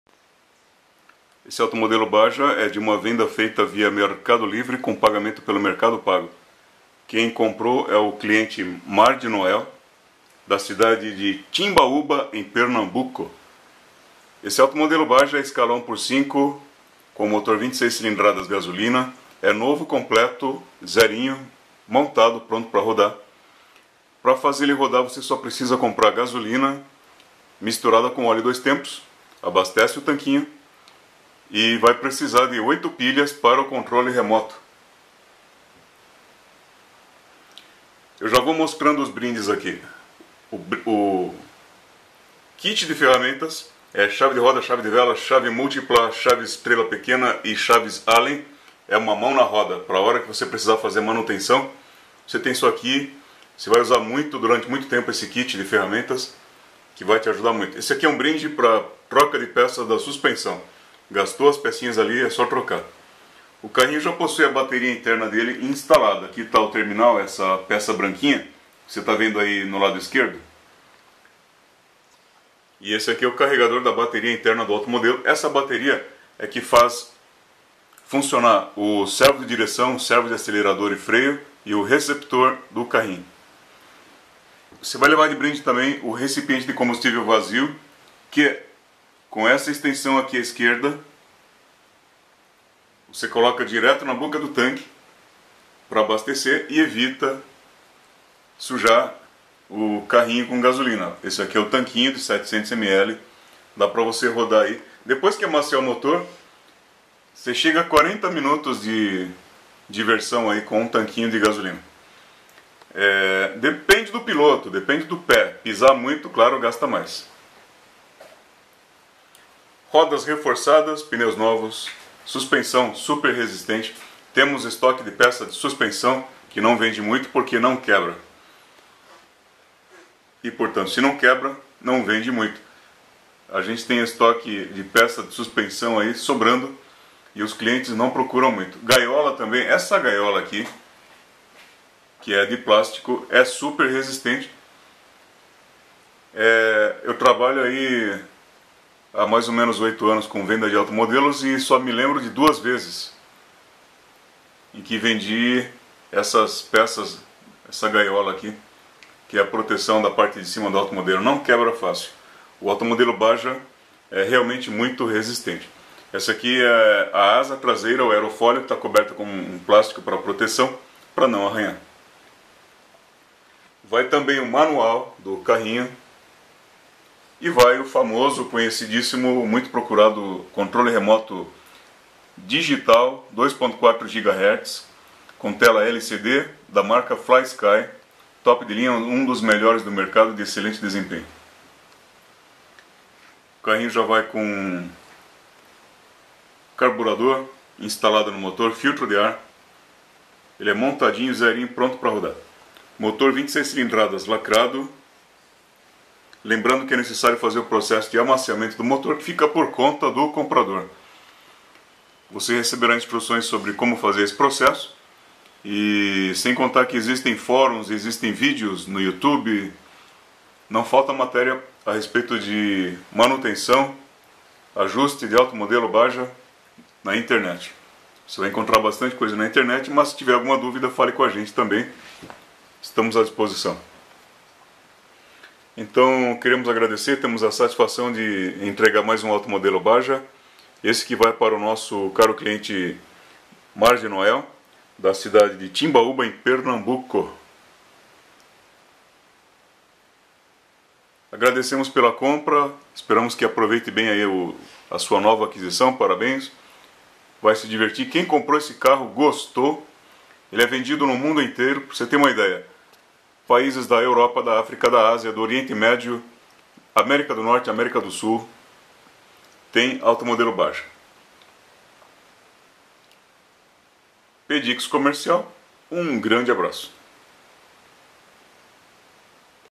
MEGA RC Automodelo SUPER RÁPIDO 1/5 Baja 26cc Gas RTR Rovan compat HPI KM dest Timbaúba PE
O Pedido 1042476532 foi fechado pelo cliente Mardnoel, de Timbaúba PE, pelo Mercado Livre / Mercado Pago, sendo um Automodelo 1/5 Baja com motor 26cc Gasolina novo, completo, montado e pronto para rodar, com controle remoto digital 2.4 Ghz Flysky de tela LCD top de linha, ferramentas e acessórios, faltando fazer só o NECESSÁRIO processo de amaciamento do motor, conforme instruções, que fica por conta do comprador. O Baja 26cc é um dos automodelos mais procurados no mercado mundial, sendo vendido em diversos países da Europa, Ásia, África, Oriente Médio, Américas do Norte e Sul. Ele possui certificação CE da Comunidade Europeia, estando portanto adequado a rigorosos padrões de qualidade.

PEDIX COMERCIAL Modelismo Profissional
Automodelos
Aeromodelos
Drones
Barcos
Lanchas
Helicópteros